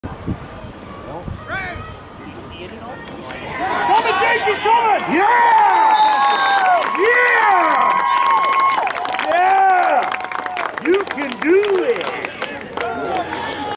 Bowles foils 3rd down fake
Football Canada Cup 2016, Game 6. Team Nova Scotia vs. Team Manitoba Silver.